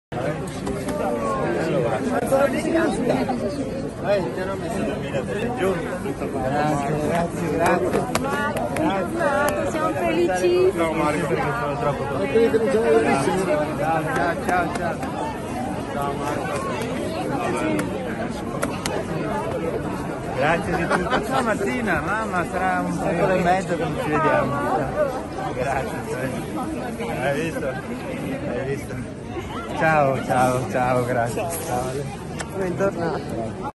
Marco Zennaro a casa, l'abbraccio della piazza di Venezia
Amici, conoscenti, parenti, cittadini comuni, rappresentanti delle istituzioni: erano un centinaio stasera in campo San Bortolomio per salutare Marco Zennaro, l'imprenditore veneziano di 46 anni rientrato sabato in Italia dopo essere rimasto bloccato per 361 giorni in Sudan. In mezzo a passanti e turisti incuriositi ("Scusate, chi è Marco?"), gli amici hanno srotolato uno dei tantissimi striscioni comparsi in questi mesi ai quattro angoli di Venezia. Tanti gli abbracci ricevuti (video di Marta Buso).